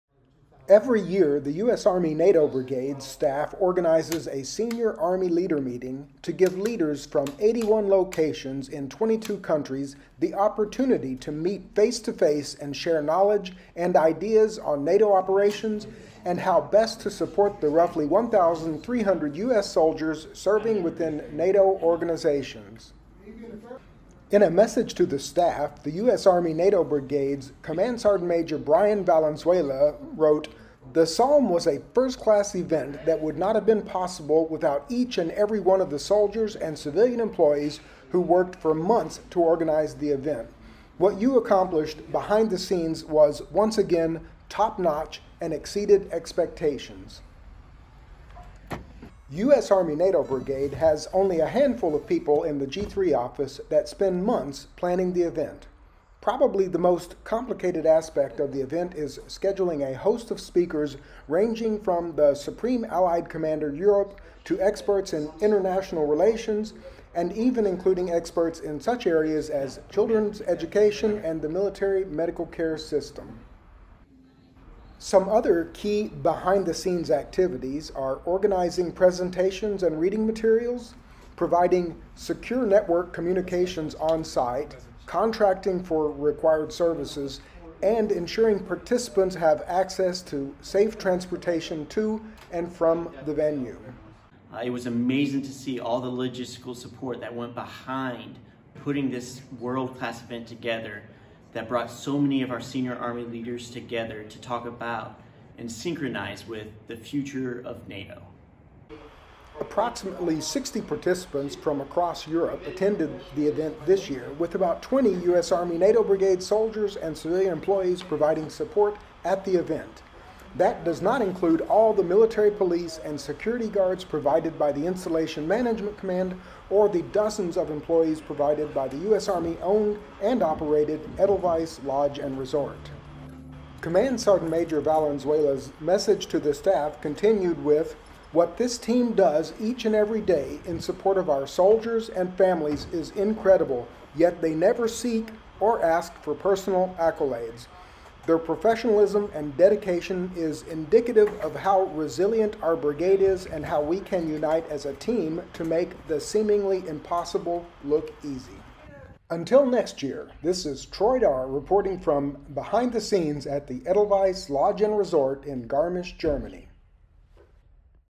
Behind the scenes at Senior Army Leader Meeting XXI
Every year, the U.S. Army NATO Brigade staff organizes a Senior Army Leader Meeting to give leaders from 81 locations in 22 countries the opportunity to meet face-to-face and share knowledge and ideas on NATO operations and how best to support the roughly 1,300 U.S. Soldiers serving within NATO organizations.

GARMISCH-PARTENKIRCHEN, BY, GERMANY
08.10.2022

Film Credits: Video by Troy Darr
U.S. Army NATO Brigade